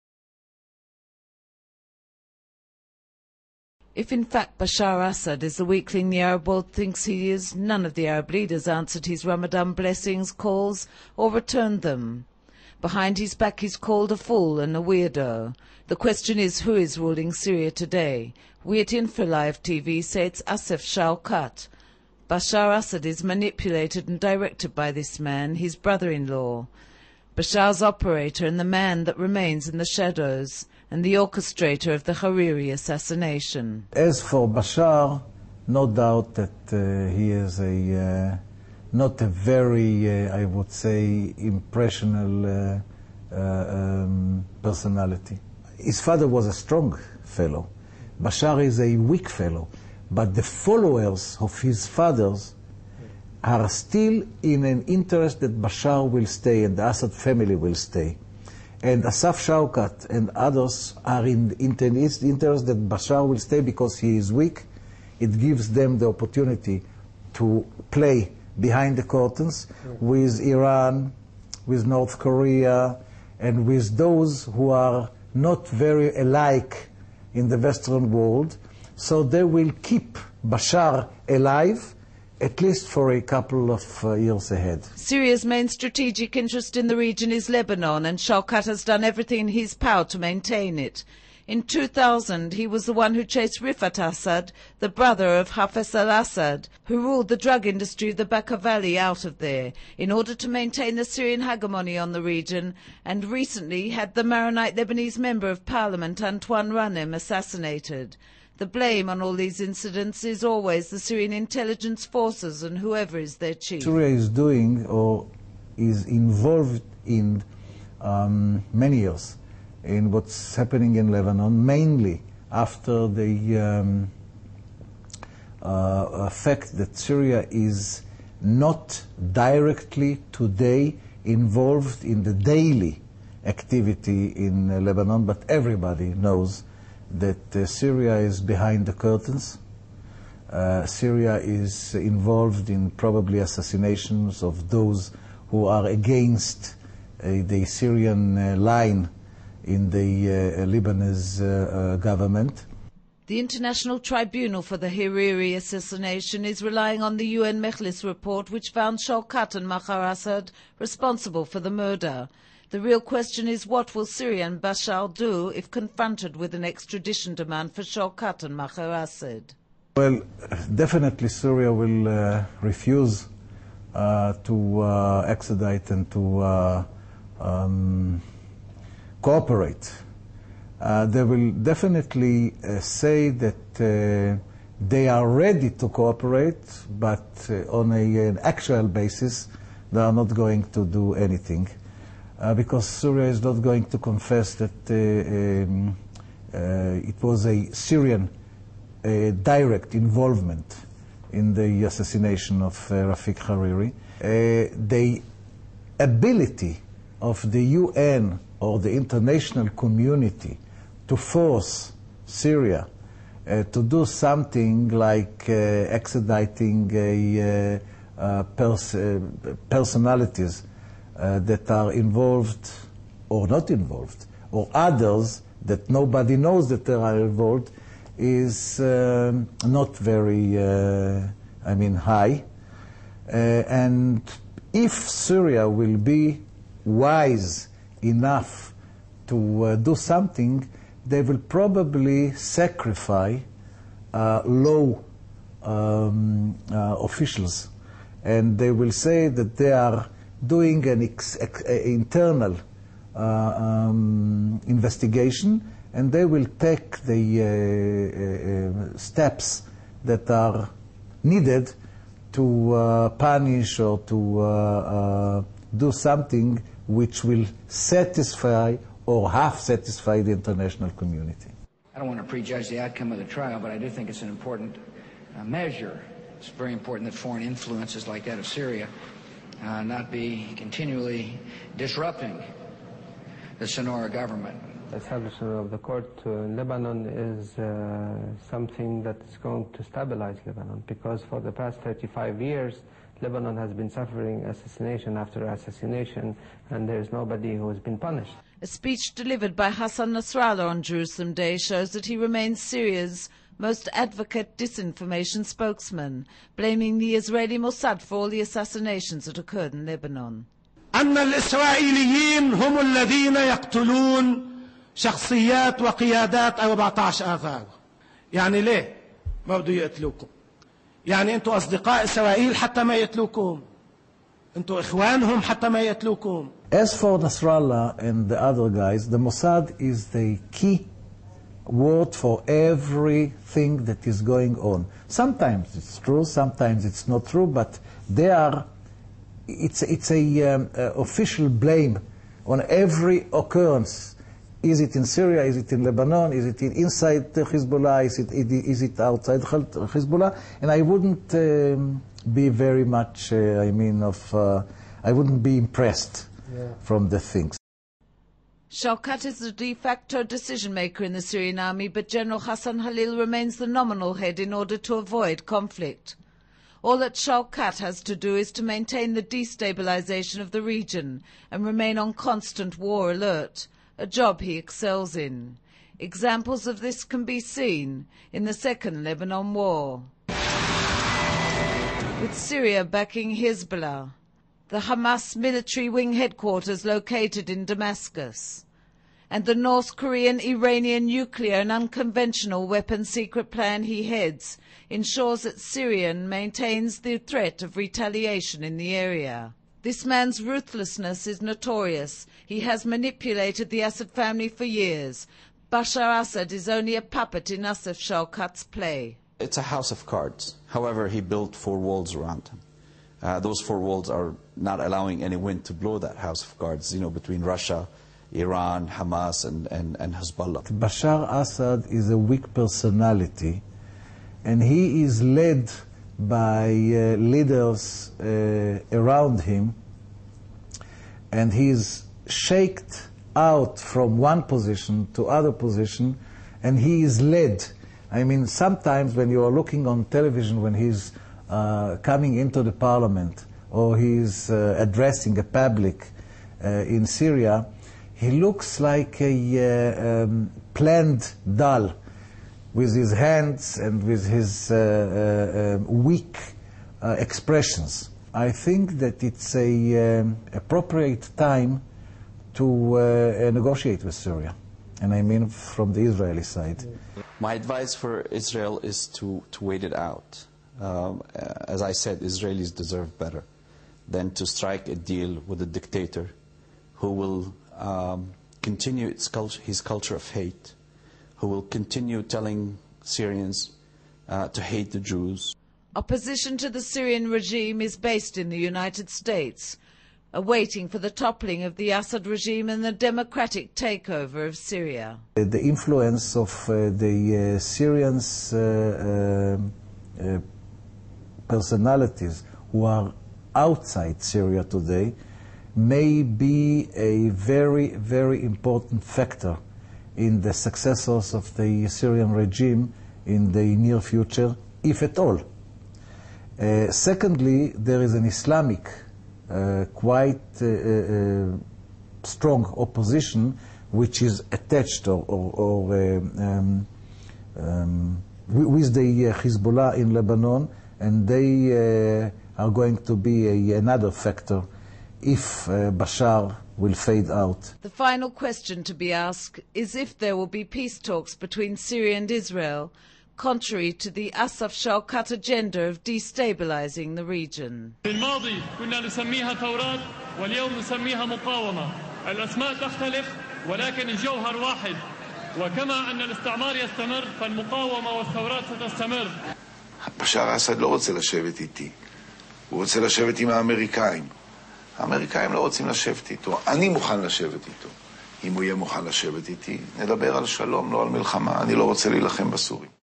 Special Report - Assef Shawkat Syria's Real Leader
Infolive.tv brings you a special in depth report on the Syrian regime and Assaf Shawkat, Syria&#39;s real leader who pulls the strings,  and orchestrated the assassination of the late Rafik Hariri. President Bashar al Assad is a puppet in Shawkat&#39;s play.07/11/07